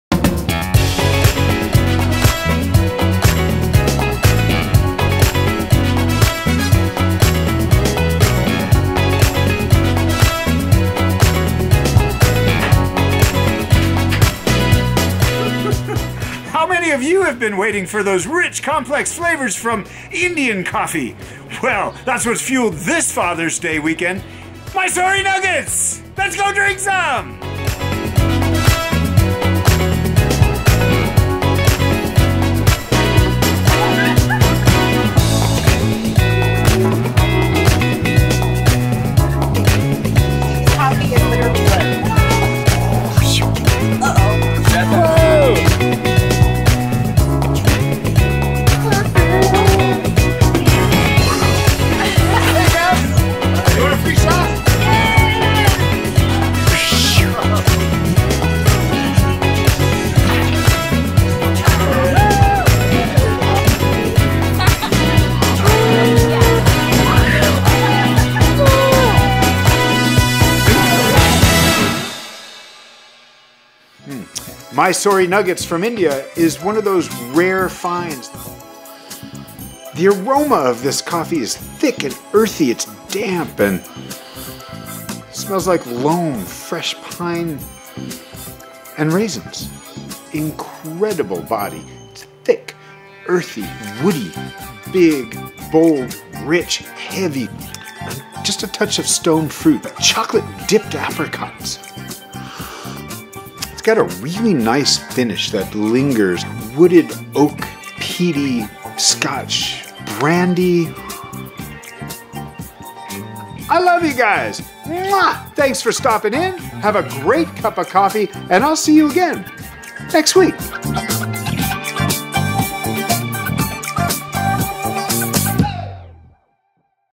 India Mysore Nuggets is Extra Bold!
How many of you have been waiting for the complex and sophisticated flavors of Indian coffee? That's what fueled us around here this past Fathers' Day Weekend and for our annual Pride celebration in Bisbee!

Hint: It's not too late to let Dad know you're thinking about him with coffee from Old Bisbee Roasters!

We have big, bold coffee from India this week: Mysore Nuggets, back after nearly three years! It's one of those rare finds.

Mysore Nuggets Extra Bold is the highest grade of Indian wet-processed Arabica coffee. The beans are classed extra bold due to the very large bean size and are as precious to fine coffee lovers as gold nuggets. Mysore is a traditional market name for coffee from south-central India. This is a rare, premium coffee and truly represents the best quality coffee from India.